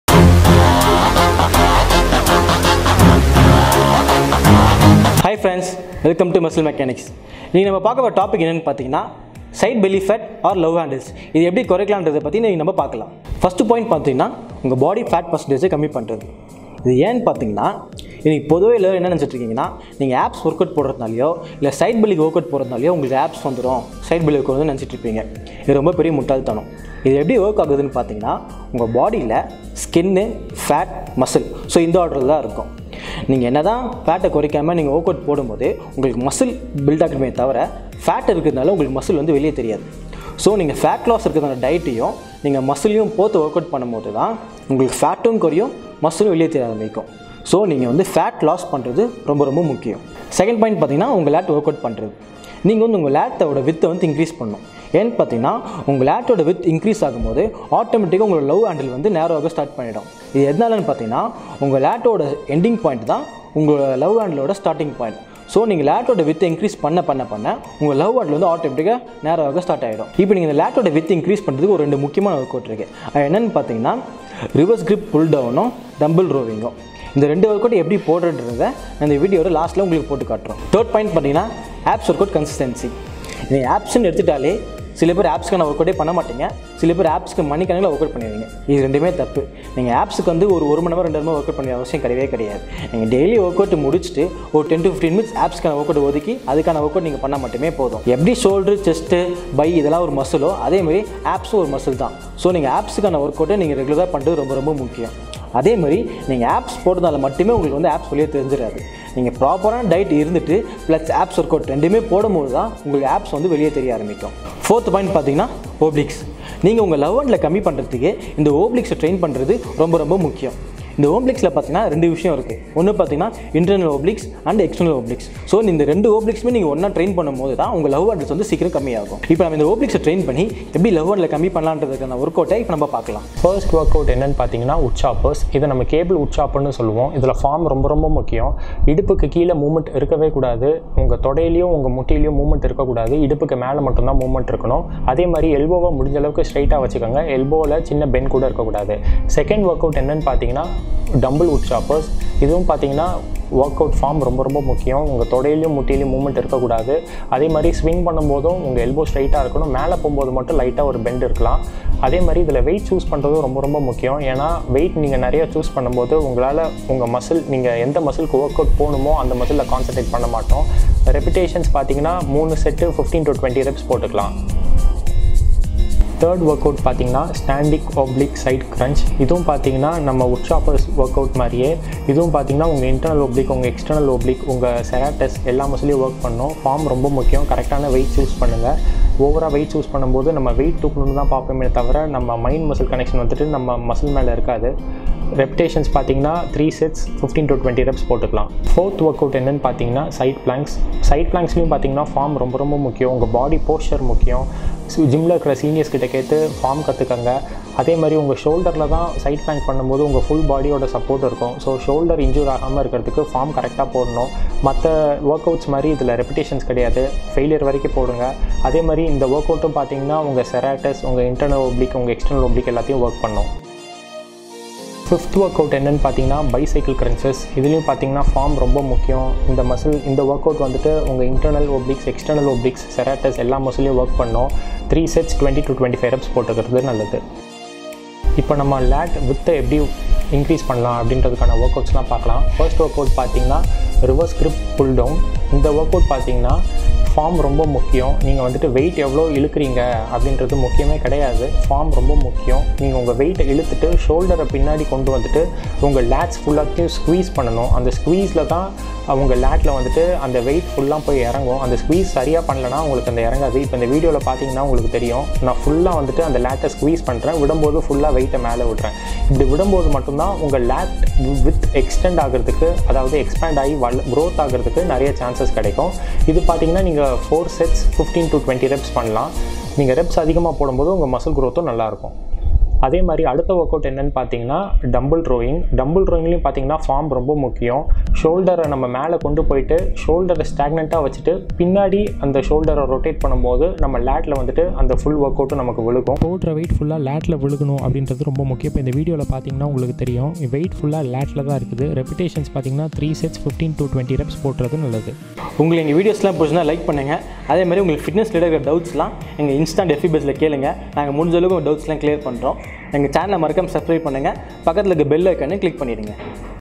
How to get rid of Love Handles | Lose Side Belly Fat Forever | Explained in Tamil
Welcome to Muscle Mechanics.

In this video, we have discussed about ways to lose your side belly fat.